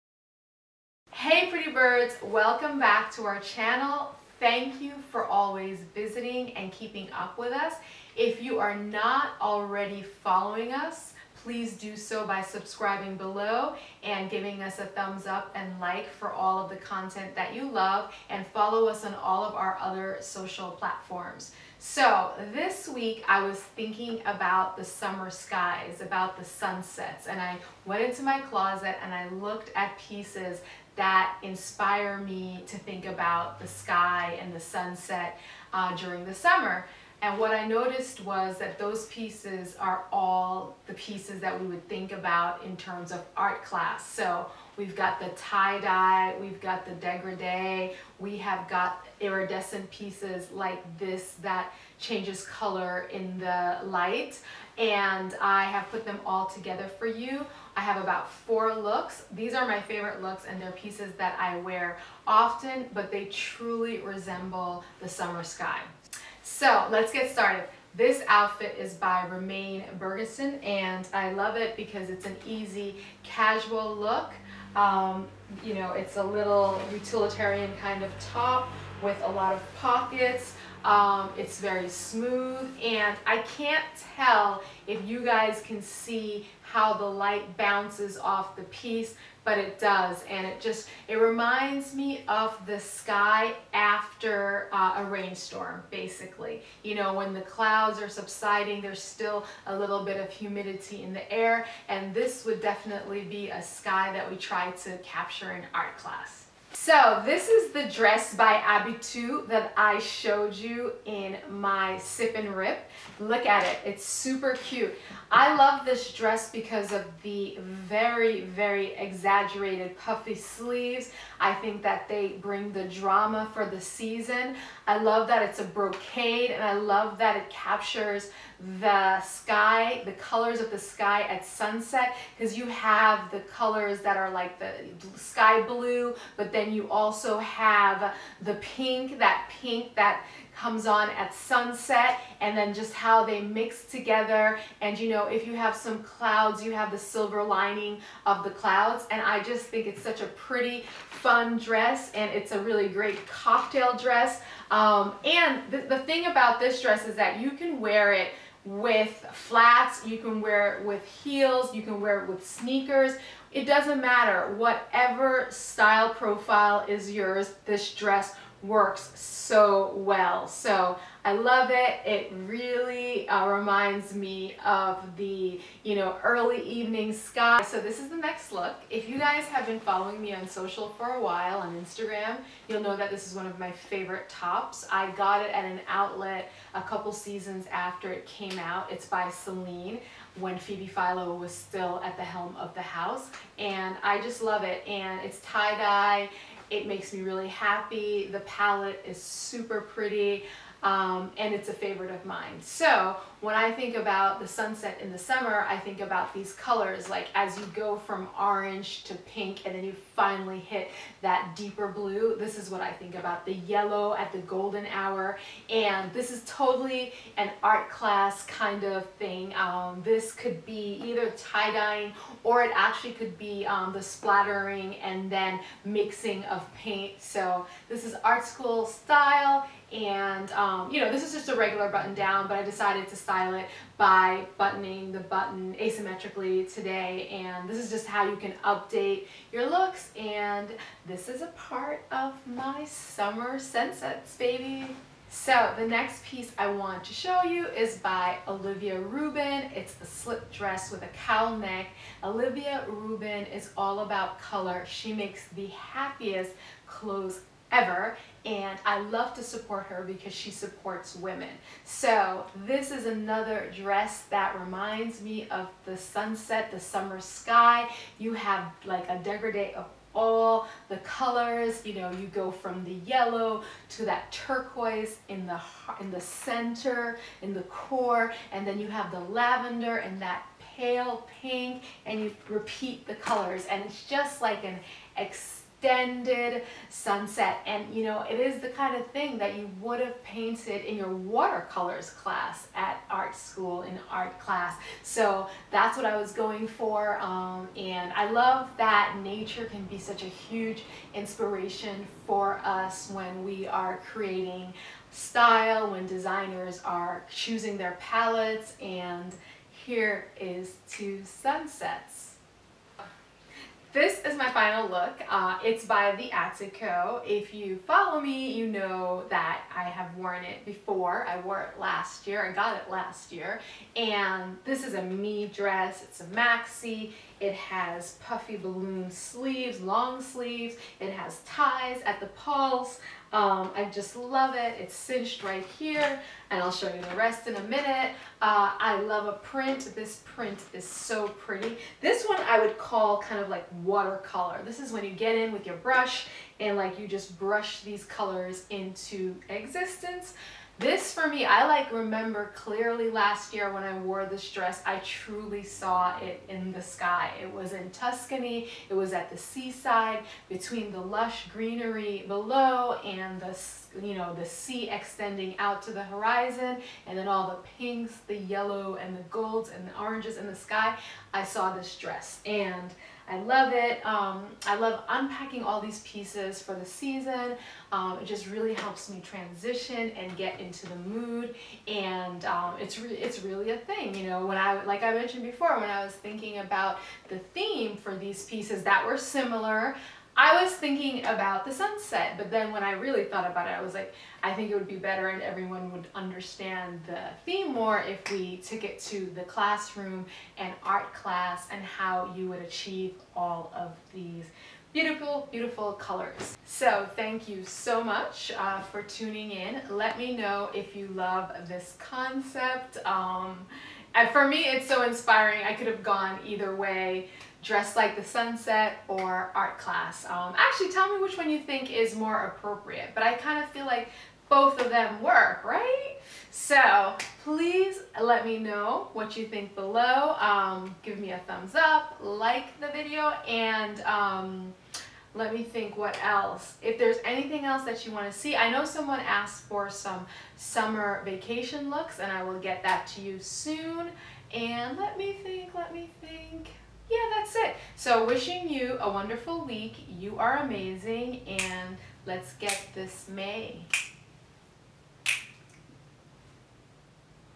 Dress Like the Sunset or How You Would Paint it in Art Class
Hey Pretty Birds,

Are you ever inspired by nature or your environment when styling an outfit?
I am especially attracted to elements that elevate my mood when planning my looks.
That’s why I mostly gravitate towards bold colors, tactile fabrics and energetic prints.

Last week while editing my closet, my eye quickly cataloged a selection of pieces that remind me of the varying stages of sunsets. From the golden hour to twilight, the sun’s light show does it for me every time: sky blue mixed with pink, golden yellow with blue and  touches of viola there in between.

Usually the prints that evoke the beauty of Mother Nature are achieved or inspired by the techniques that we learned in art class or arts and crafts: water color, tie-n-dye, papier-mâché, drip technique and glitter and glue projects. Hence my indecision with the title of this week's style diary entry: Dress Like the Sunset or How You Would Paint it in Art Class. Both parts of the title are relevant for me, what do you think? Tell me below in the comments.

So as we transition further into our Covid-19 norm and we can move outside of our homes, gardens and beyond our neighborhoods to enjoy the sunsets; will you dress like one? If so, here are 5 looks to set your sights on!

Look 1: Silver Lining, soft skies after a rainstorm.

Look 2: At Sunset.

Look 3: The Sunset When Mother Nature is Showing Off.

Look 4: The Romantic Sunset Blend.

Look 5: The Seaside Sunset that Keeps Giving.